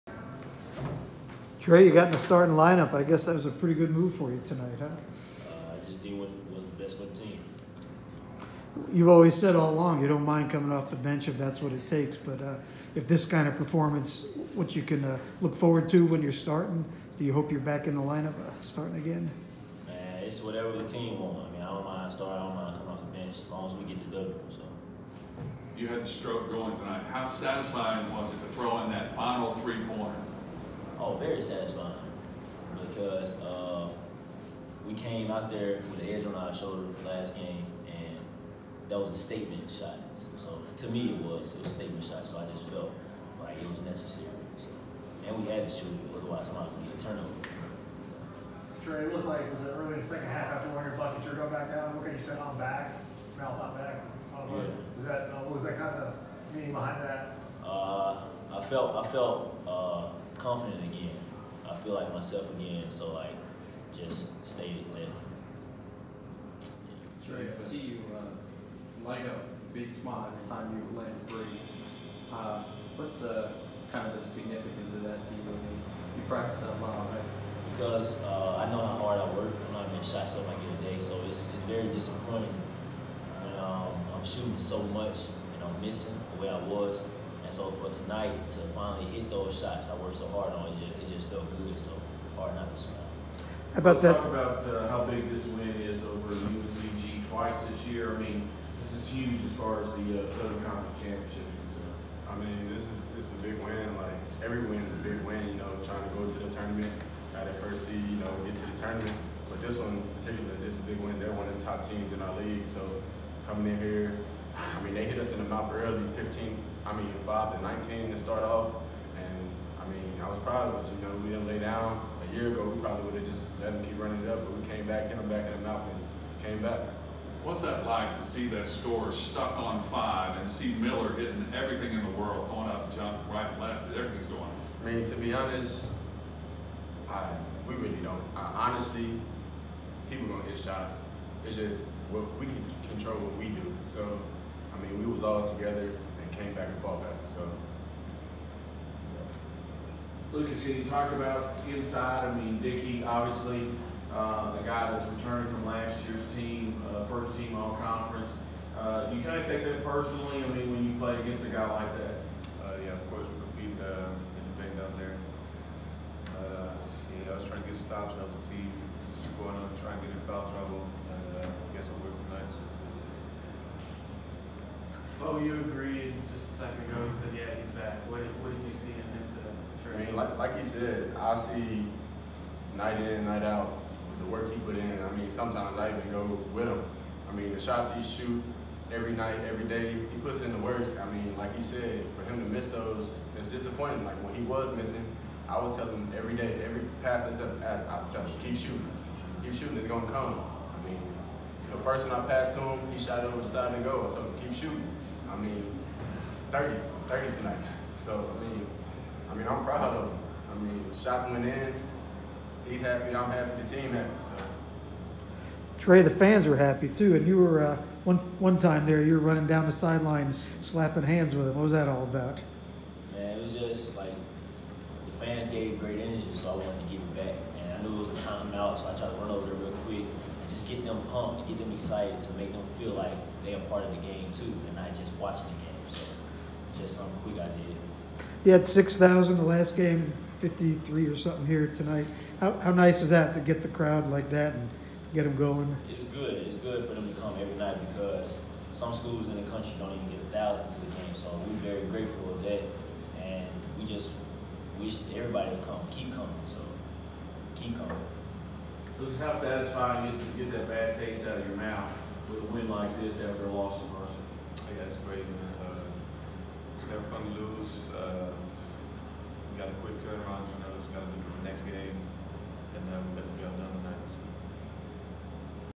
ETSU’s Tray Boyd, Bo Hodges and Lucas N’Guessan
After big win over UNCG